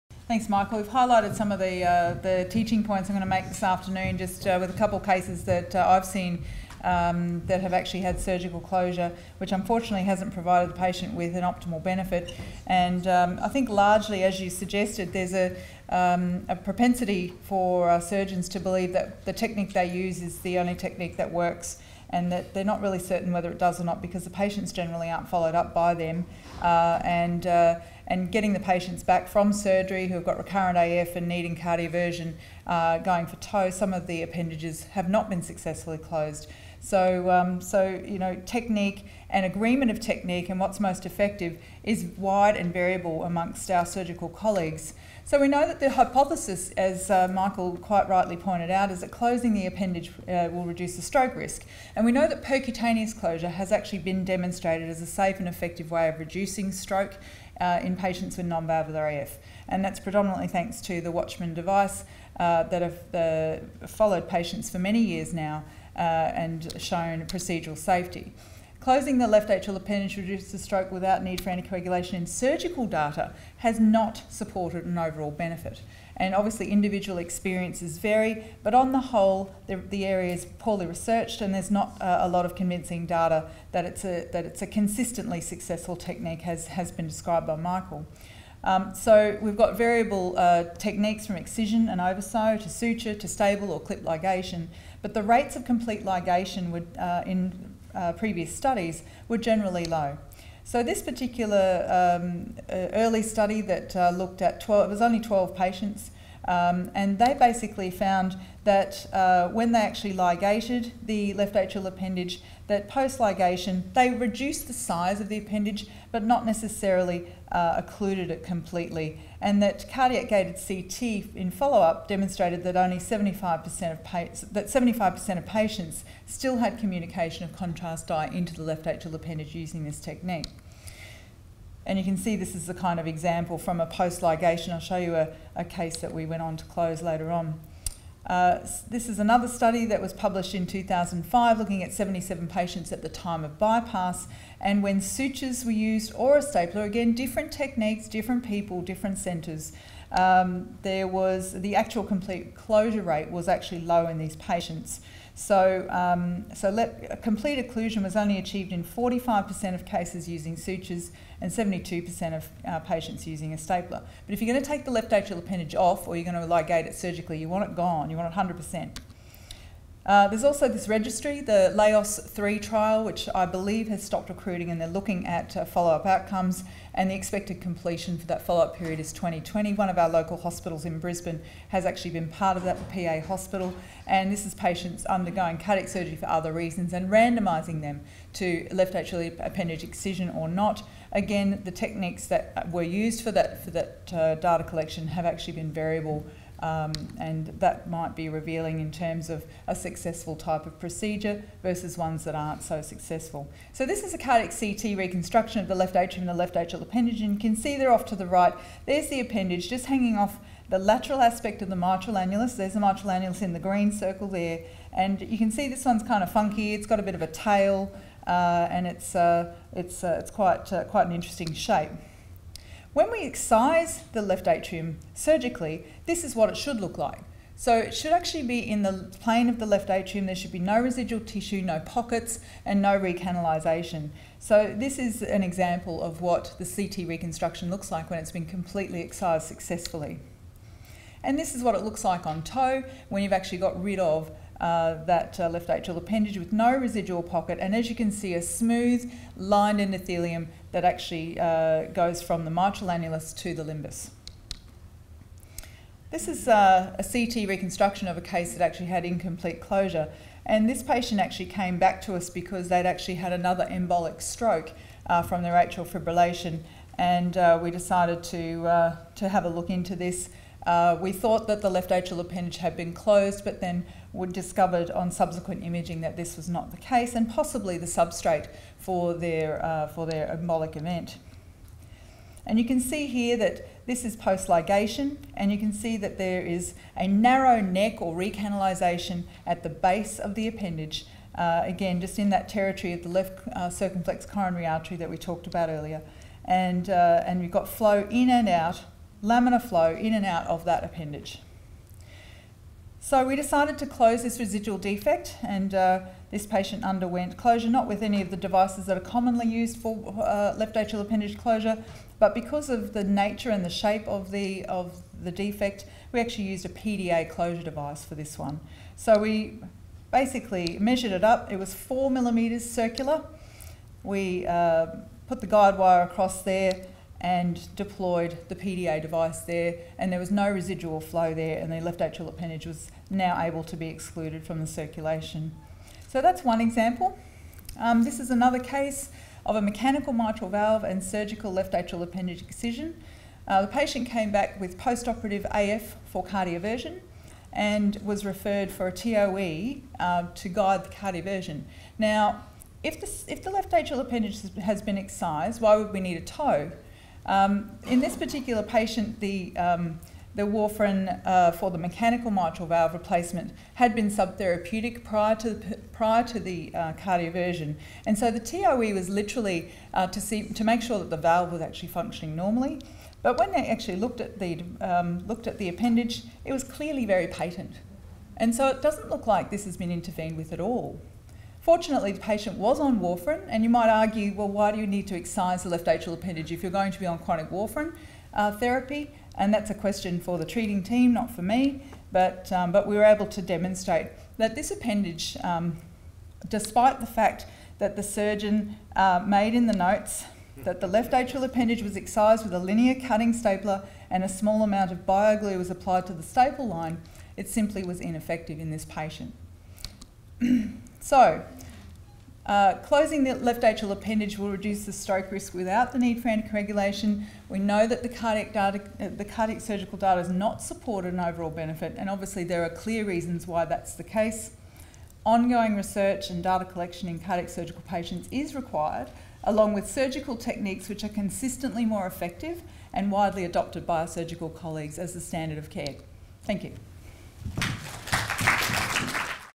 Left Atrial Appendage “Non” Closure - Dr Julie Humphries
Dr Julie Humphries, Cardiologist at The Prince Charles Hospital, talks about the possibility of incomplete left atrial appendage closure and consequences.

Presented at the Structural Heart Disease Australia Left Atrium Symposium on 20th July 2019.